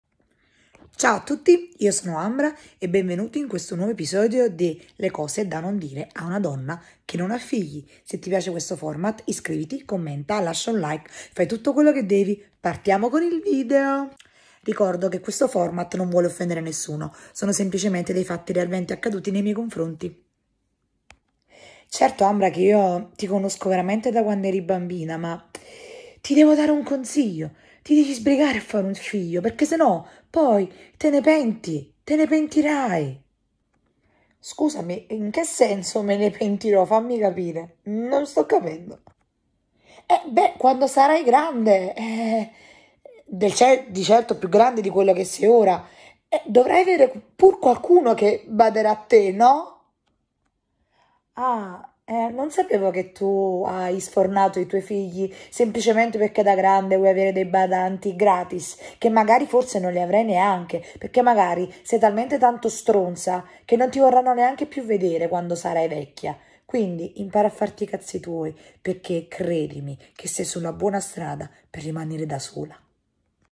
Cose da non dire a una donna che non ha figli nuovo episodio
QUESTO FORMAT NASCE PER SENSIBILIZZARE LE PERSONE A ESSERE MENO SUPERFICIALI E A CAPIRE CHE LA PERSONA CHE HANNO DI FRONTE È FATTA DI CARNE OSSA ED EMOZIONI CHE SPESSO VENGONO FERITE DA FRASI FUTILI E STUPIDE
CHIEDO SOLO UN PO’ DI RISPETTO E DI UMANITÀ